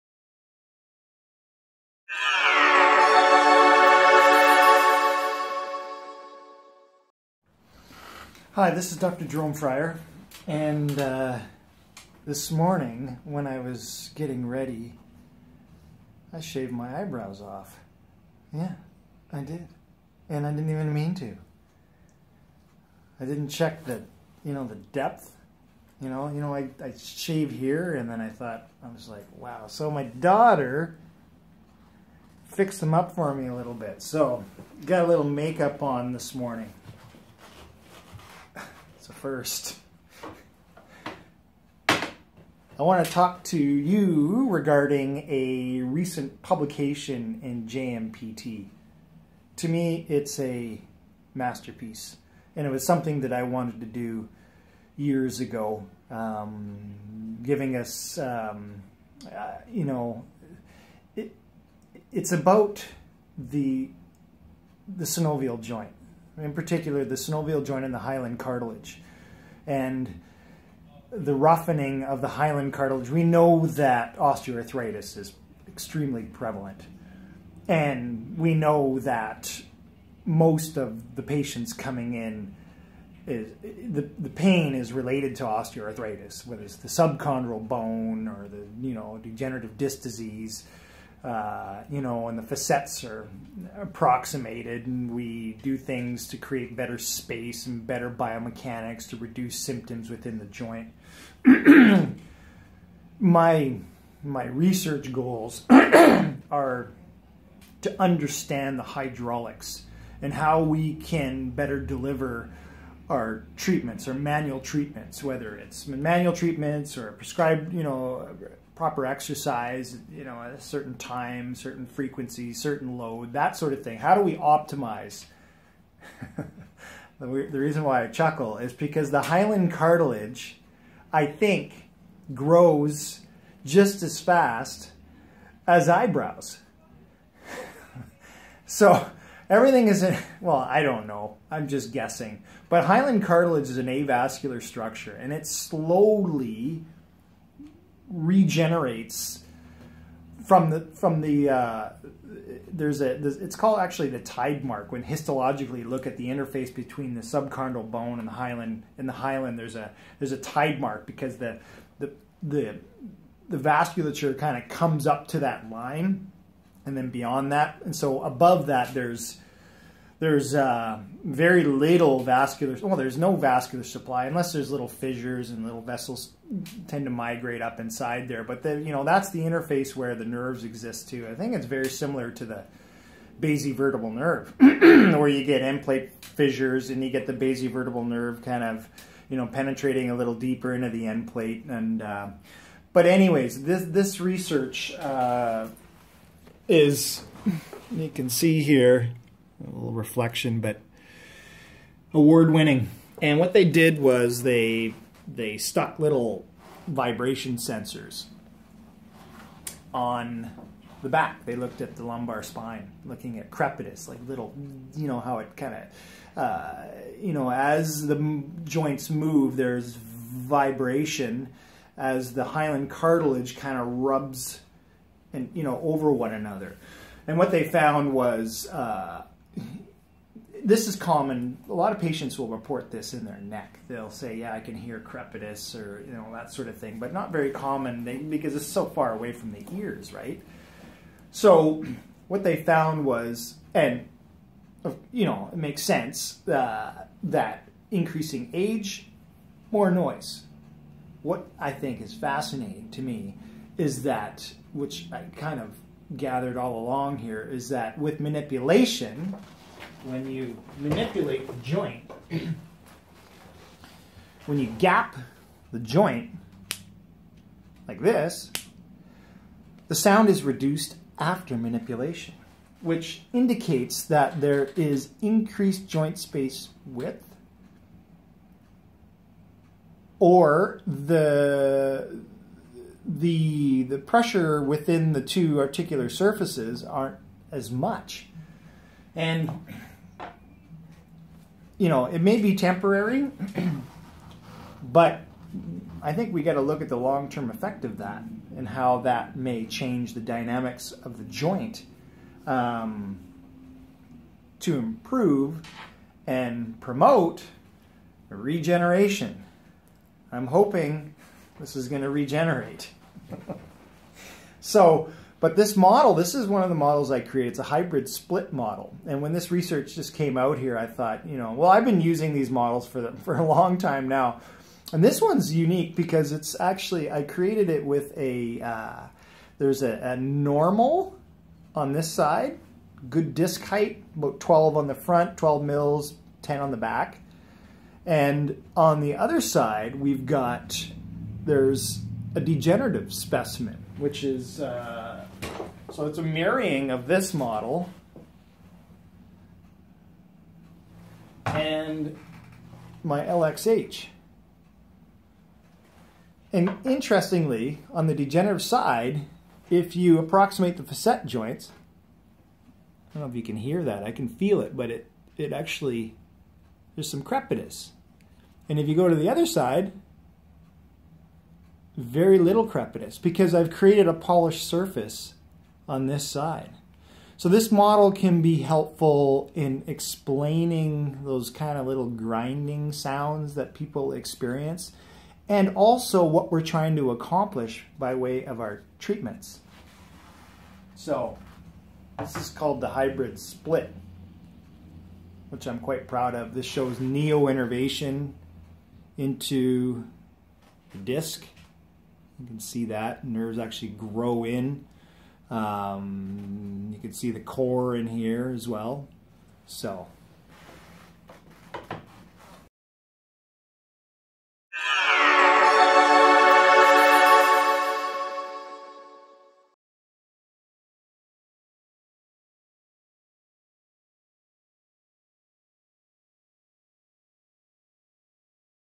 Crepitus, Sound and Manipulation - Joint Science
Sounds are common in and around synovial joints. What are they telling us? These group of researchers looked at the vibration and noises of the z-joints of the lumbar spine. Very interesting and cornerstone work. Congratulations!